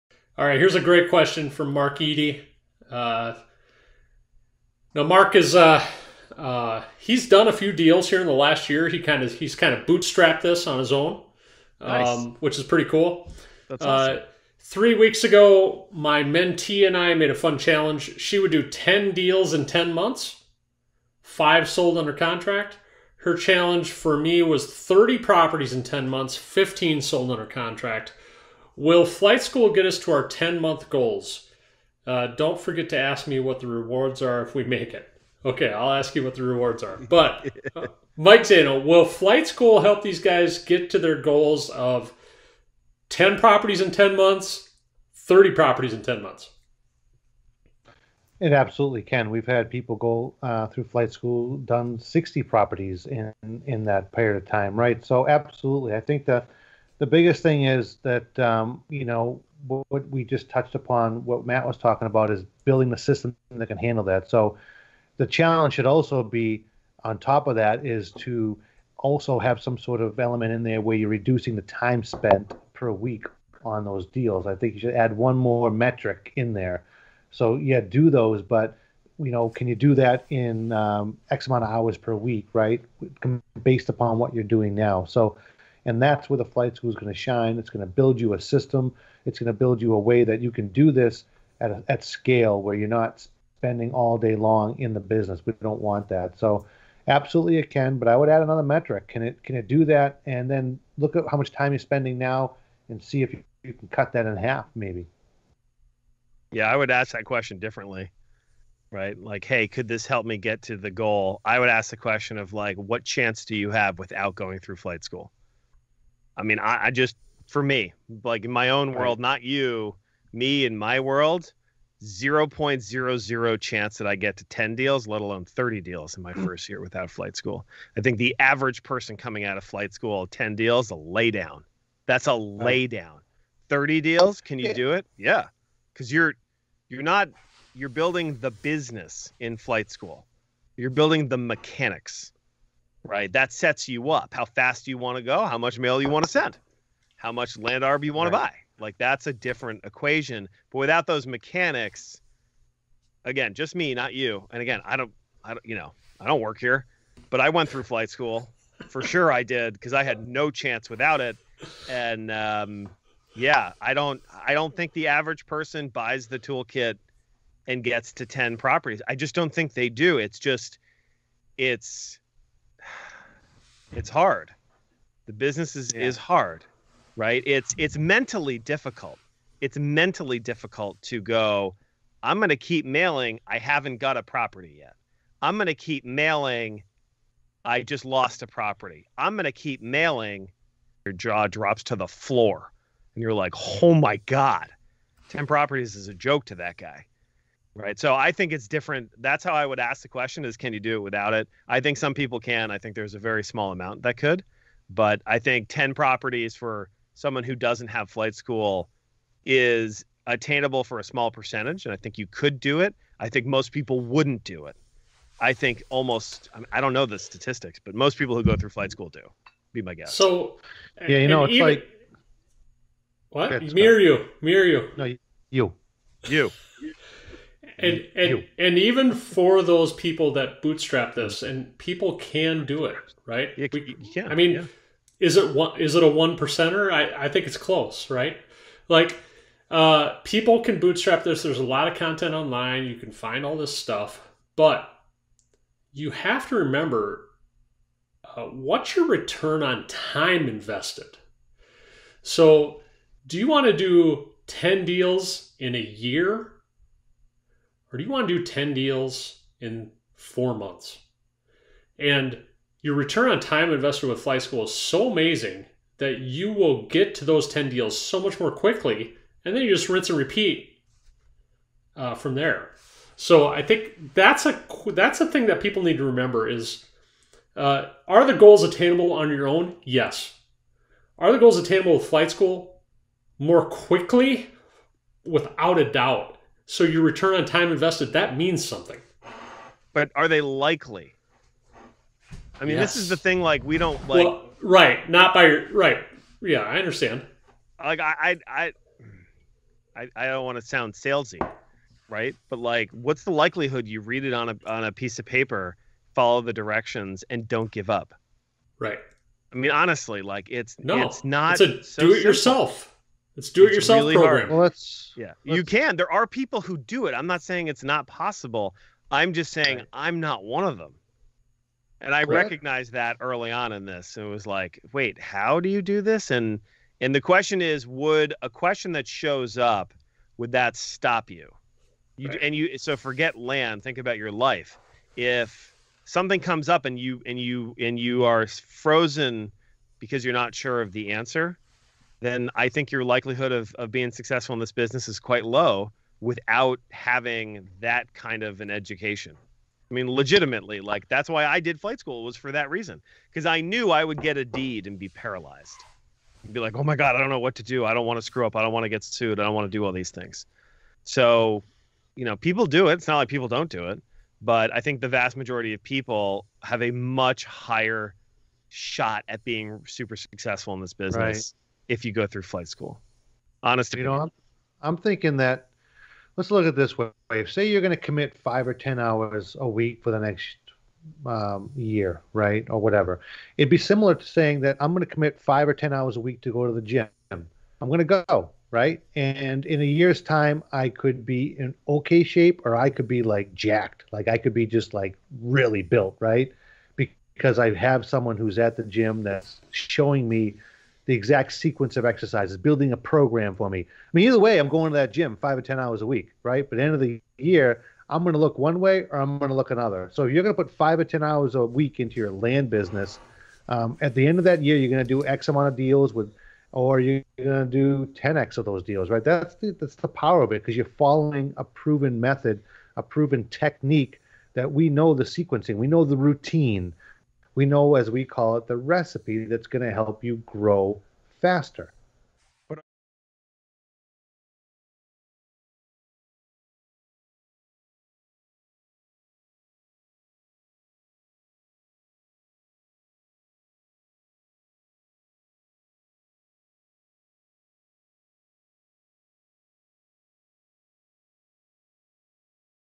How to Scale Your Land Business Faster
Listen in as the Nite Cap guys discuss how to scale your land business faster by going through The Land Geek Flight School. Doing Flight School will teach you how to build the system that you need to grow your land business faster.